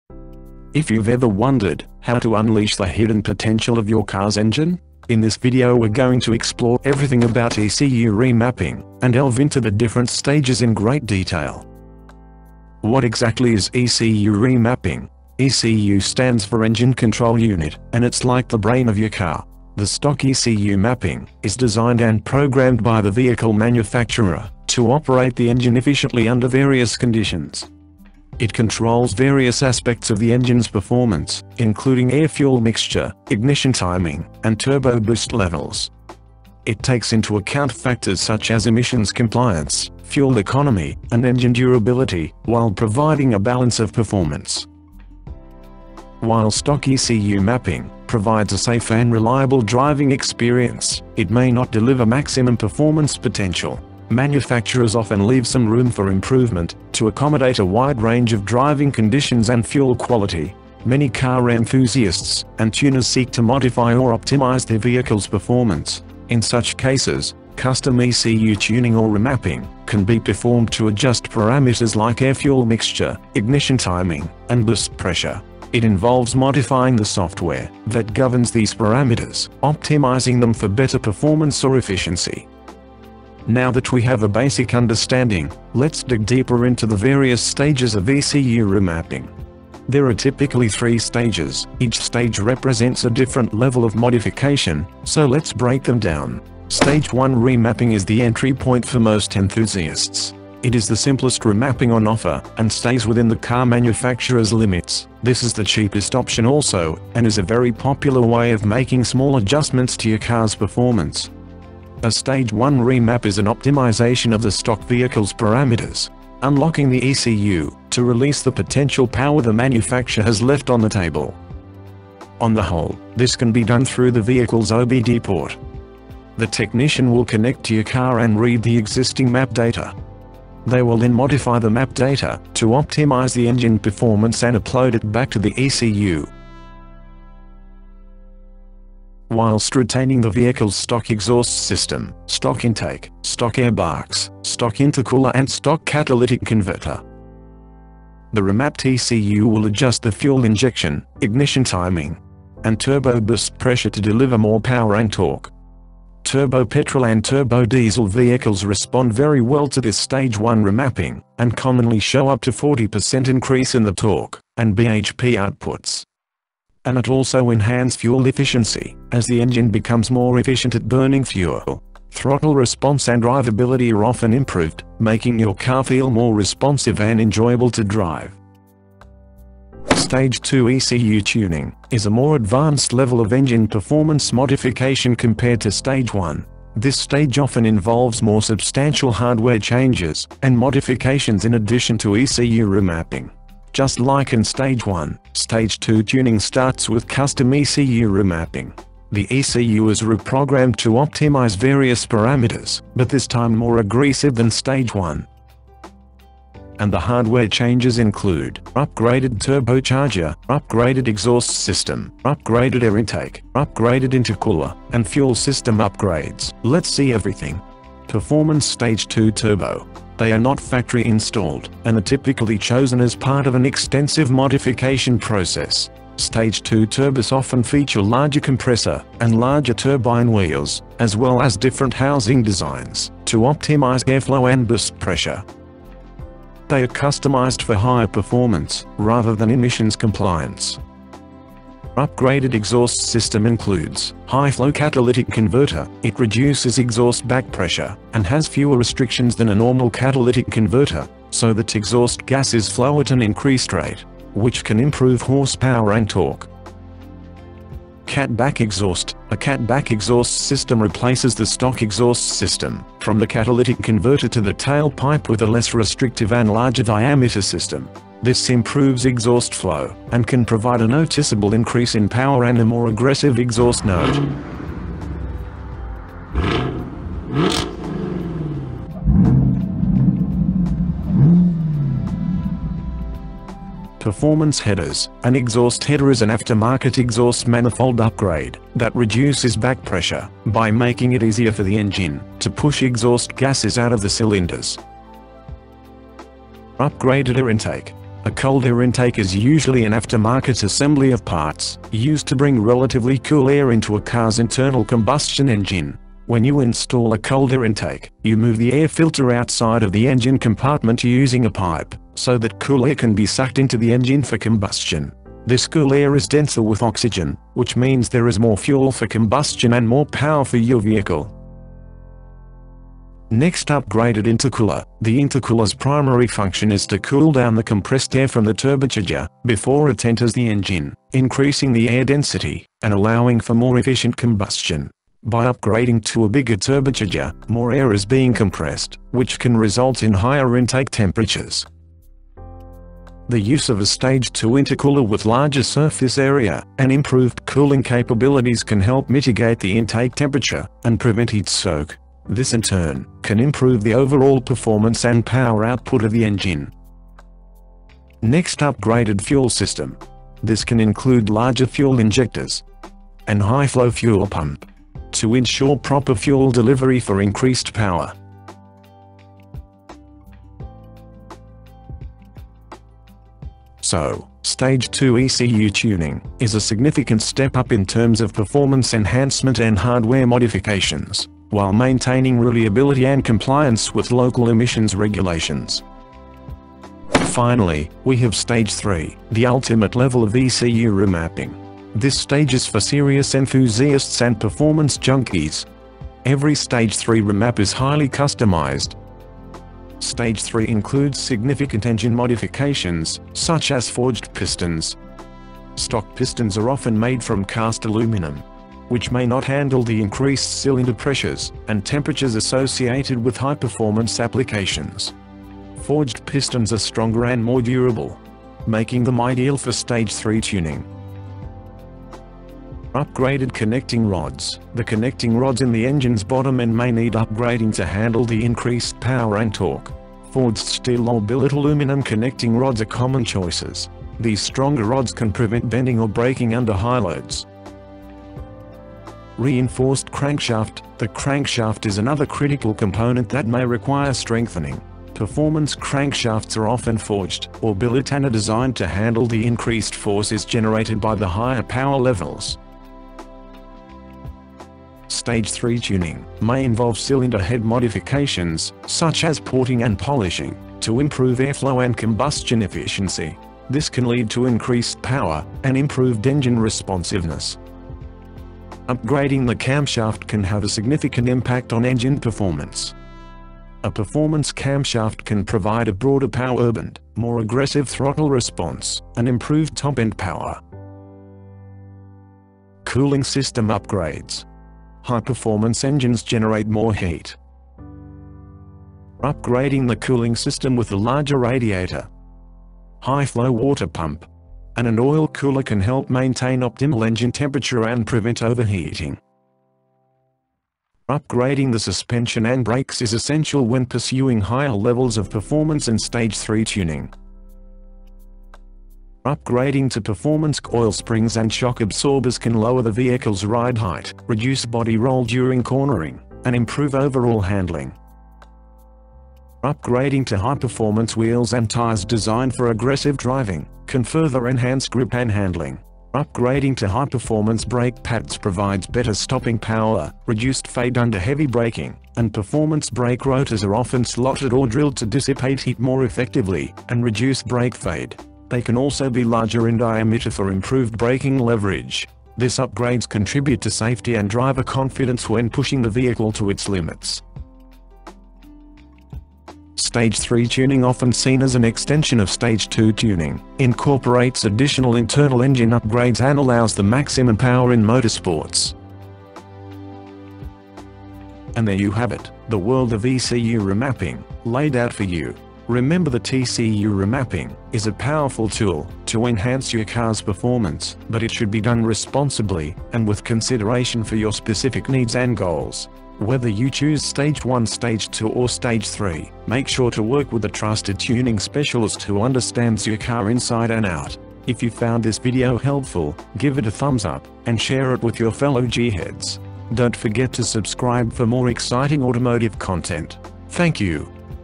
ECU/Engine Remapping- Every STAGES & Mods, Increase in Power Explained
What is ECU remapping/Engine Tuning/Race Tuning? how it works, and the different stages of remapping. Discover the power of customizing your vehicle's engine performance and gain insight into the hardware upgrades that can elevate your driving experience.

We break down the fundamentals of ECU remap, from the intricacies of recalibrating engine parameters to optimizing fuel efficiency and power delivery. Learn about the essential stages of remapping, including Stage 1, Stage 2, and Stage 3, and the significant performance gains they offer.

We explore the role of hardware upgrades in ECU remapping, discussing the importance of high-performance air filters, exhaust systems, and intercoolers. Find out how these components work with ECU tuning to maximize power, throttle response, and fuel efficiency.

Whether you're a car enthusiast or a newcomer to ECU remap, this video offers valuable insights to help you make informed decisions about tuning your vehicle. Join us as we provide a step-by-step explanation of the ECU remapping process and uncover the benefits of unleashing your engine's full potential.